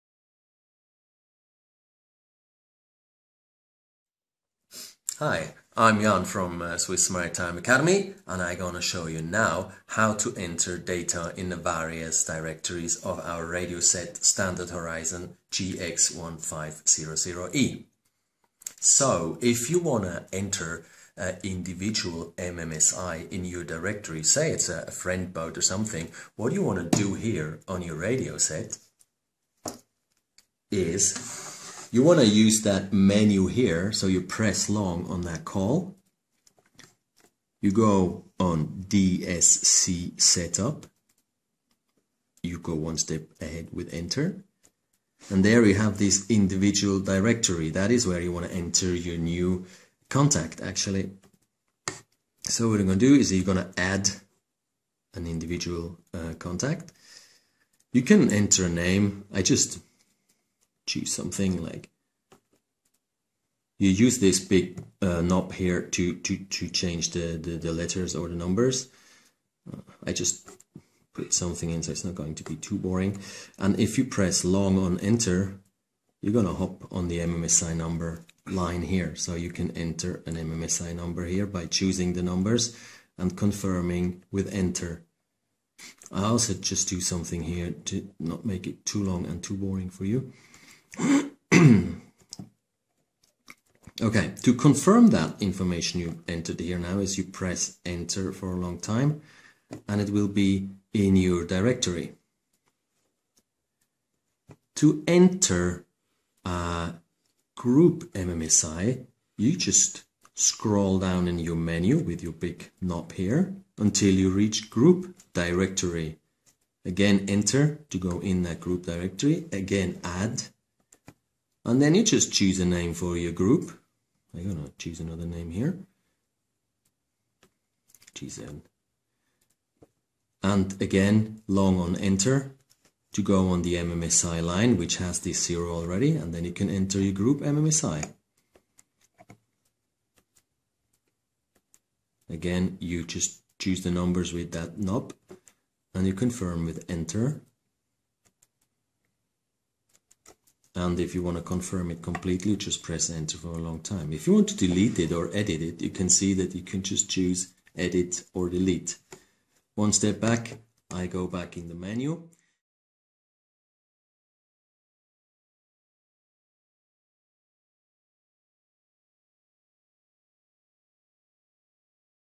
SRC-Seefunk-DSC-Directories
Jan from Swiss Maritime Academy explains how to enter data in the various directories of a Standard Horizon GX1500E for your familiarisation with the radio set used for SRC Seefunk exam.